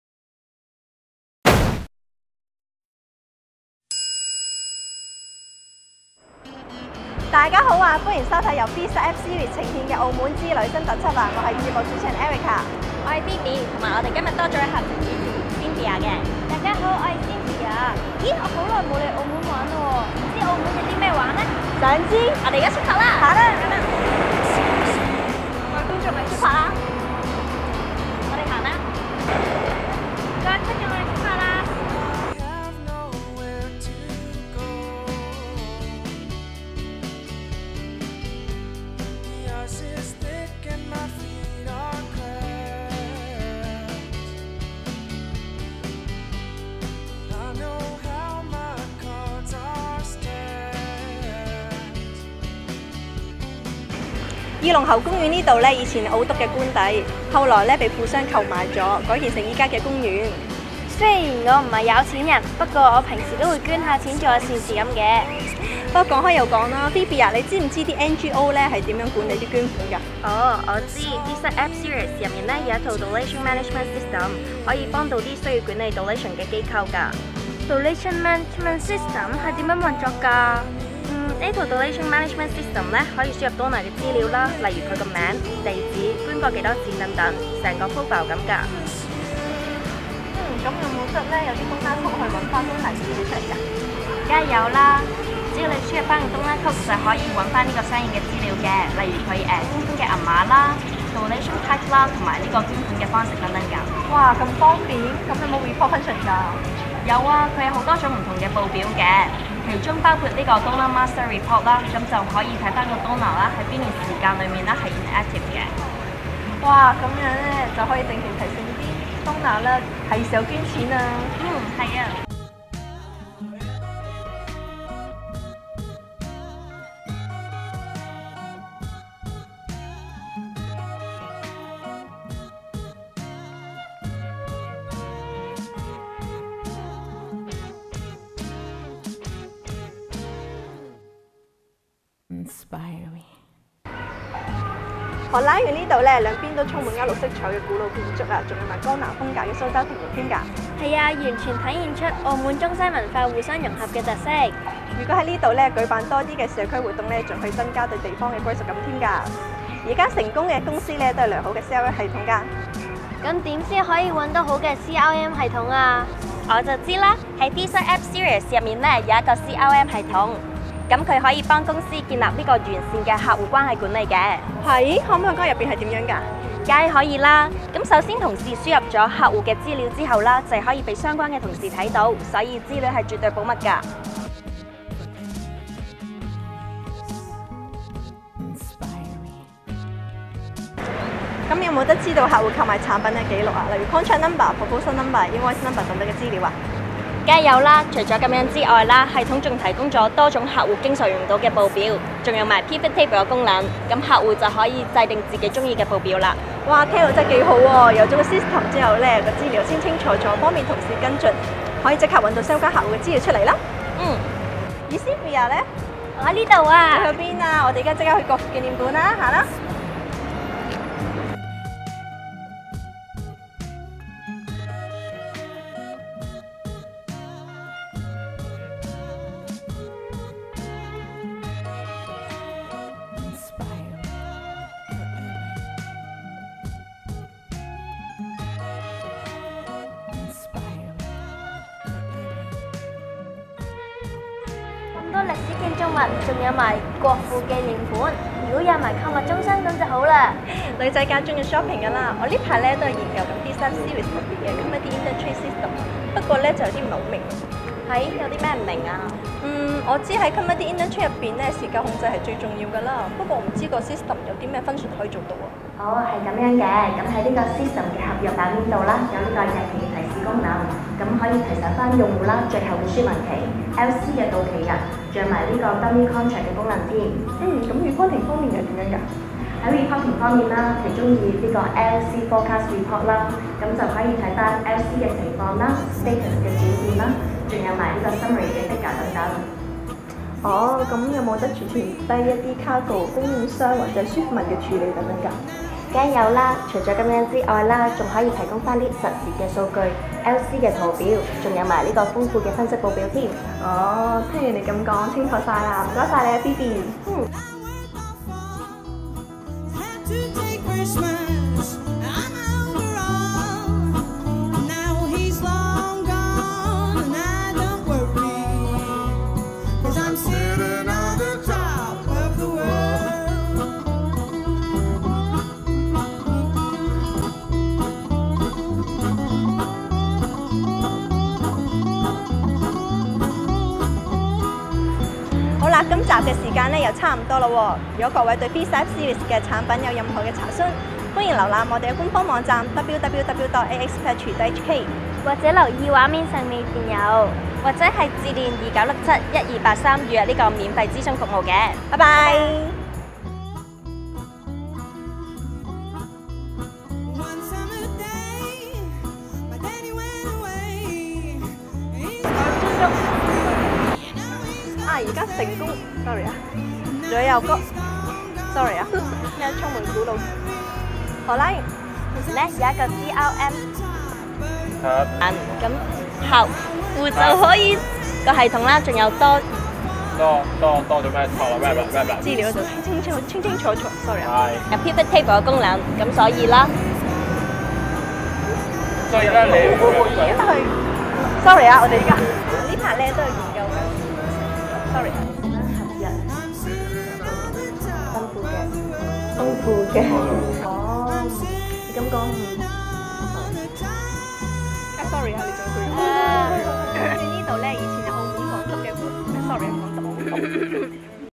FESAapp 呈獻: 澳門之旅特輯 Part 1
各位觀眾，YourBizIT Channel 今次為大家帶來新系列 - FESAapp 呈獻: 澳門之旅特輯，有 Erika 和 Phoebe 帶我們到澳門為大家主持節目，介紹 FESAapp Series 產品，包括: Customer Relationship Management System，Donation Management System，Commodity Indent Trade System ，其間更會暢遊澳門名勝. 想知更多？快 Click 入短片吧！